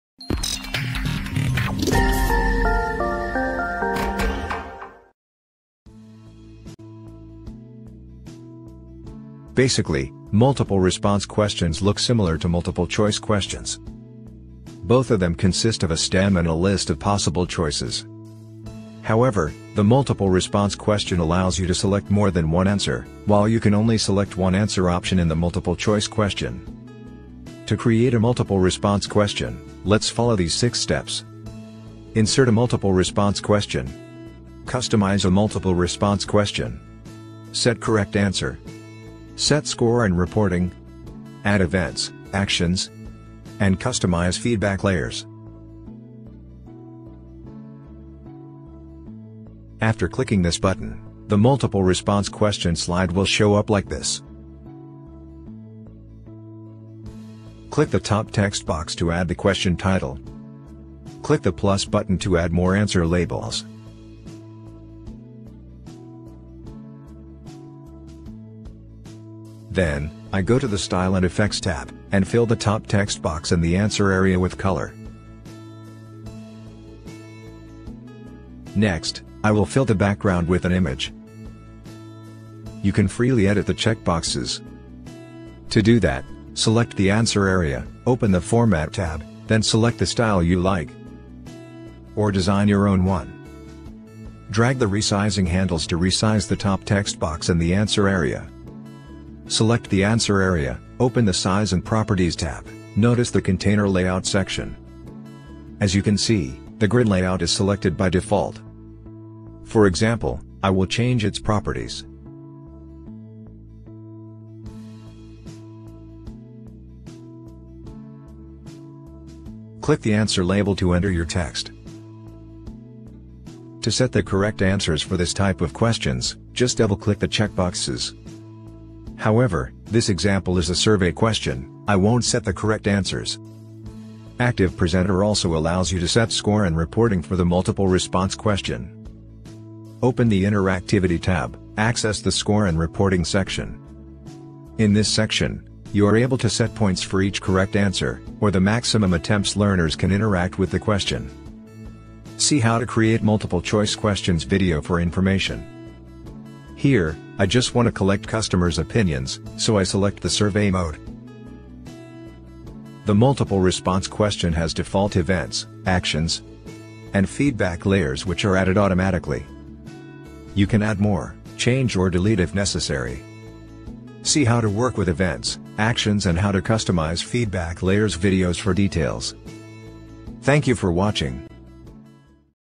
How to Create Multiple Response Questions - ActivePresenter 8
Basically, Multiple Response questions look similar to Multiple Choice questions. However, the Multiple Response question allows you to select more than one answer, while you can only select one answer option in the Multiple Choice question. In this video:
0:48 Insert a Multiple Response Question
0:55 Customize a Multiple Response Question
2:04 Set Correct Answers
2:16 Set Score & Reporting